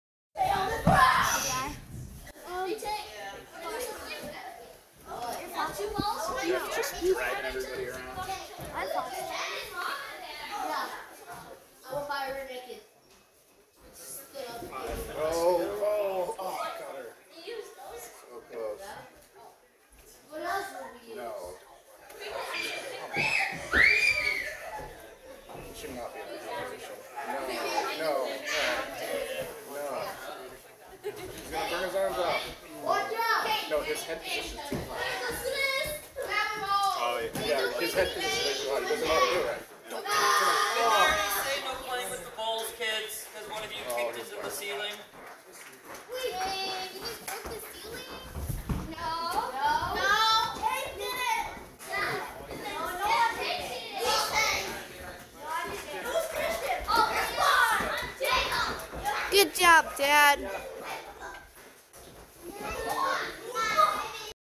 Cutter vs. Mohammed Frostbite Vale-tudo Summer smoker June 2023
Last half of my roll. Cheerleader sweep attempt at 0:14 tore my knee up. Tried to push through the pain, but was too distracted and immediately gave up mount and tapped to a shoulder attack from a head in arm attempt.